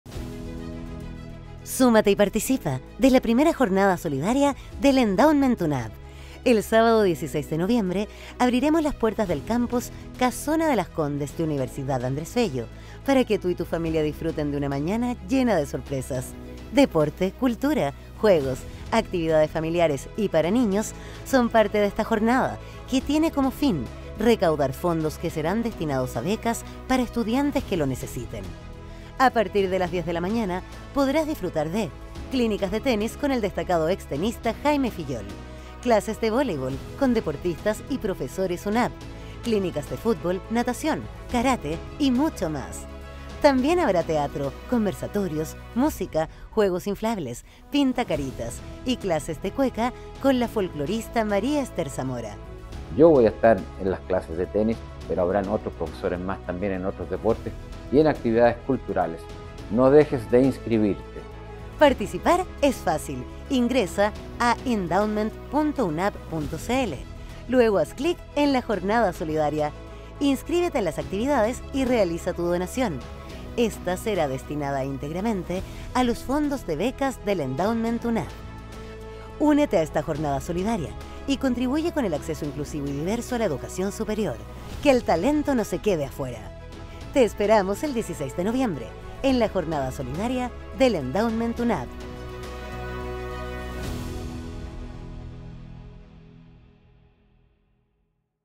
Participa de la Jornada Solidaria del Endowment UNAB 2024
Te invitamos a ser parte de esta jornada solidaria que busca recaudar fondos para la entrega de becas a estudiantes que lo necesitan. La cita es para el sábado 16 de noviembre a partir de las 10.00 hrs en el Campus Casona de Las Condes de UNAB.